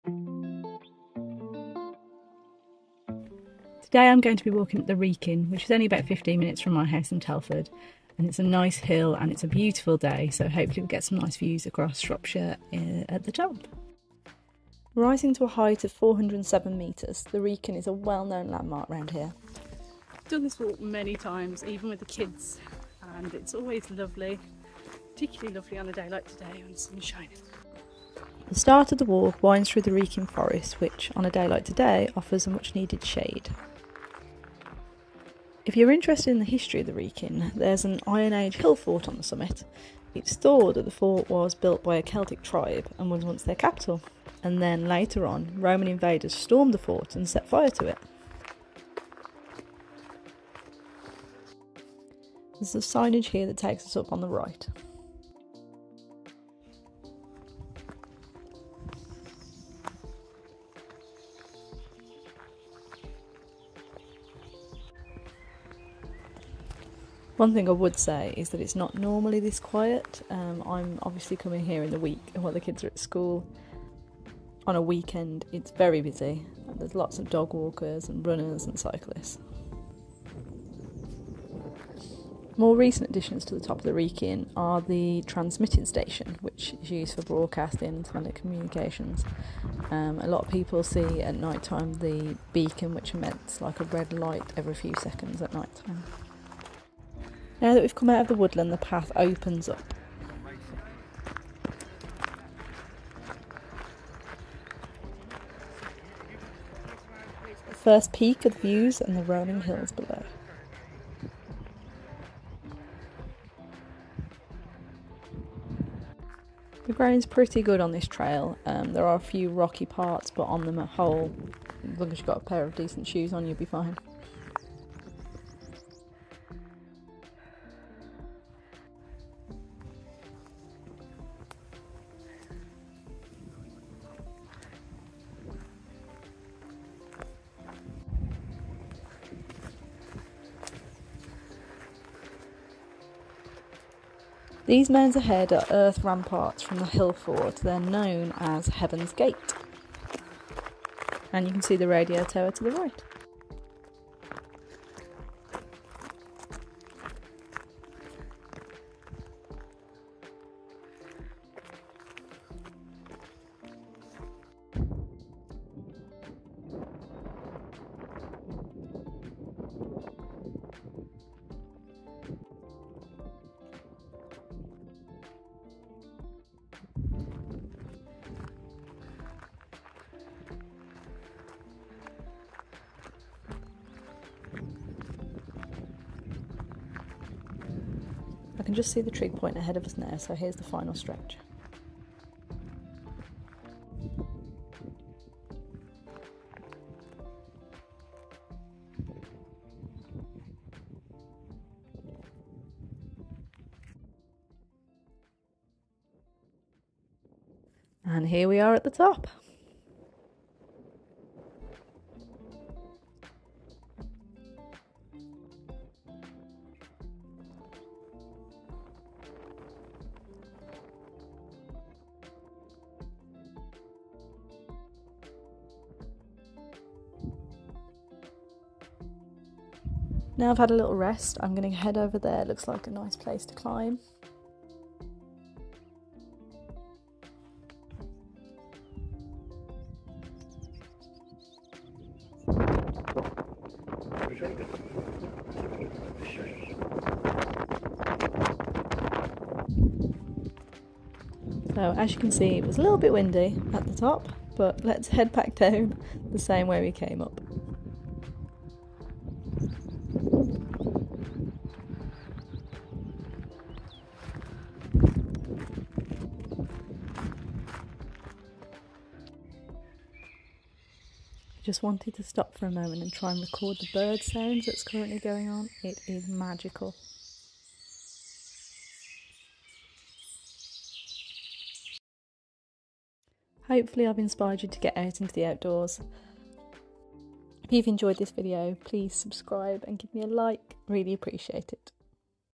The Wrekin | Solo Walk | Exploring Shropshire
Walk with me up The Wrekin, a hill in Shropshire.

Rising to a height of 407 metres The Wrekin is a prominent and well-known landmark. If you've been to the Midlands and ever heard the phrase ‘going round the Wrekin’ the phrase may finally make sense.

There is an Iron Age hill fort on the summit. It is thought the fort was built by a Celtic tribe and was once their capital and then later on Roman invaders stormed the fort and set fire to it.

A more recent addition is the Wrekin transmitting station, used for broadcasting and telecommunications. At the top of the main mast is a beacon which emits a red pulse of light every few seconds at night.

I picked a lovely day for it, hopefully my adventures will inspire you to get out and about too. Particularly for the Moms out there like me, who enjoy a bit of me time in nature.

Comment if there's anywhere you think I should visit. I would love to find some new places to explore.